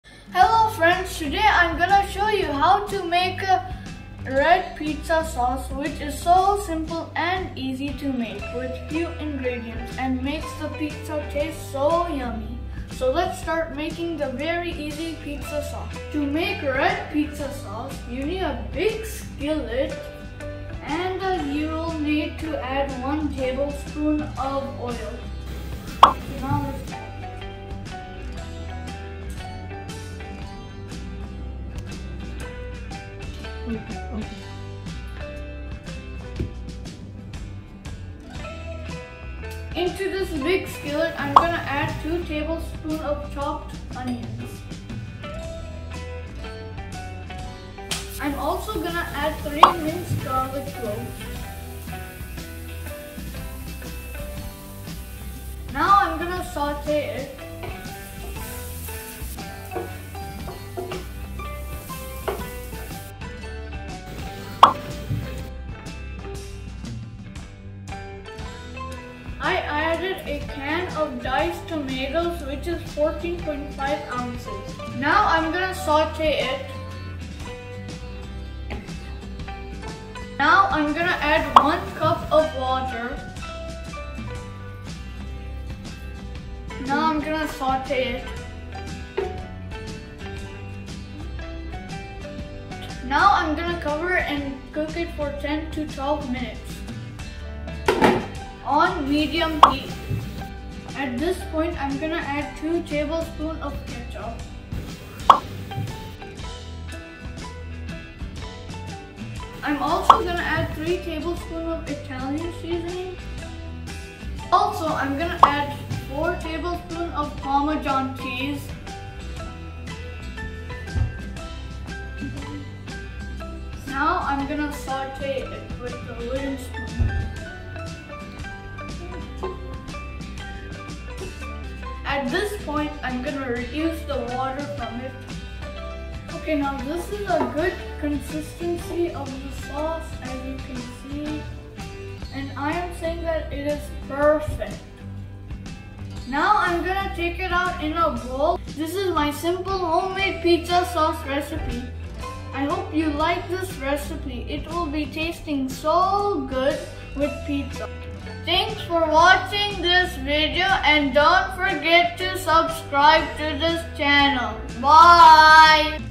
Red Pizza Sauce Recipe From Scratch | Pizza Sauce Recipe By Tabs Yummy Recipes
This red pizza sauce recipe from scratch is so simple and quick to make and it tastes absolutely delicious. With few simple ingredients, this pizza sauce is ready in no time. So, friends, if you are wondering how to make easy homemade red pizza sauce from scratch and used few simple ingredients like ketchup and diced tomatoes, watch this full video. This easy and homemade red pizza sauce recipe includes some diced tomatoes, chopped onions and minced garlic along with ketchup, Italian seasoning and parmesan cheese to enhance its flavor to the next level for any pizza. This red sauce can be used for any pizza such as vegetable pizza, cheese pizza, pepperoni pizza you name it. So please try this simple and delicious homemade pizza sauce and enjoy it with the family and friends.